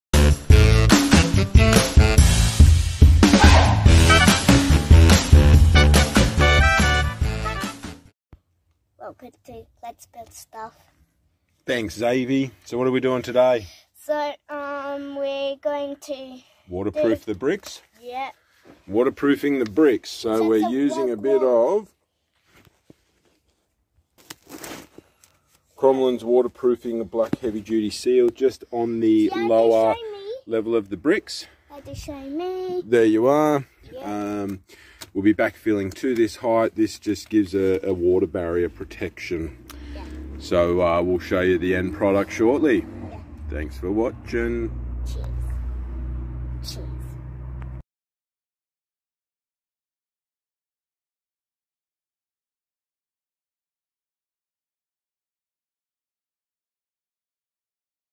Waterproofing the bricks
sealing up the lower bricks before we backfill